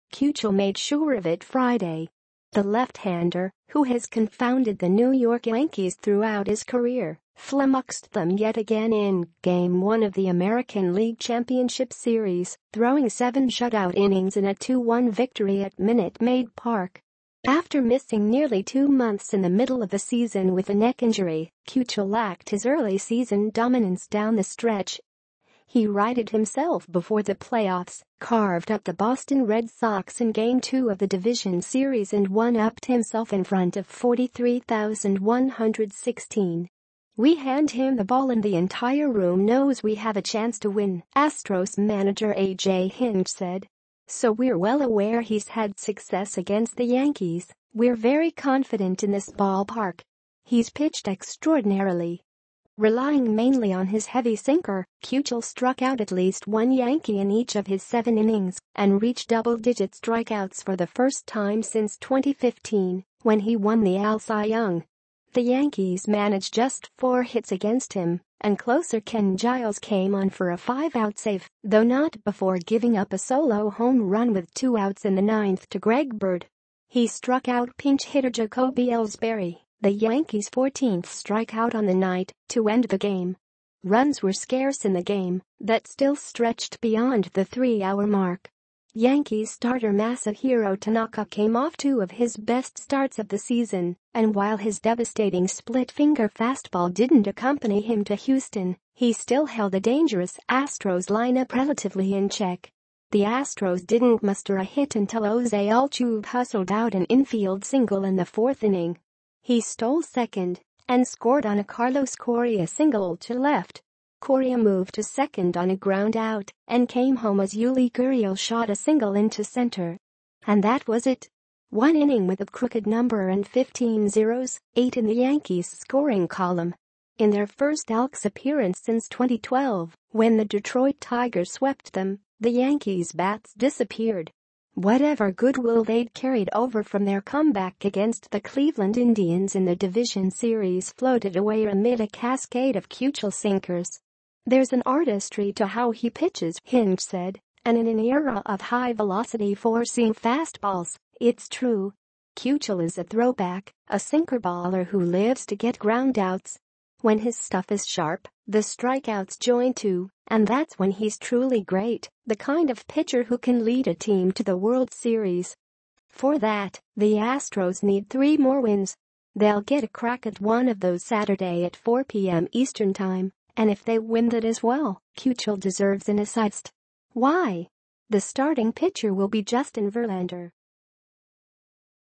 ALCS Game 1: Dallas Keuchel stymies Yankees in Astros' 2-1 victory
As midnight approached on Aug. 31 and Justin Verlander weighed whether to accept a trade to the Houston Astros, he received a phone call from Dallas Keuchel. The Astros ace wanted to give Verlander that last push toward saying yes, and though the call was quick, he punctuated it with words Verlander wouldn’t soon forget: “You won’t regret this.”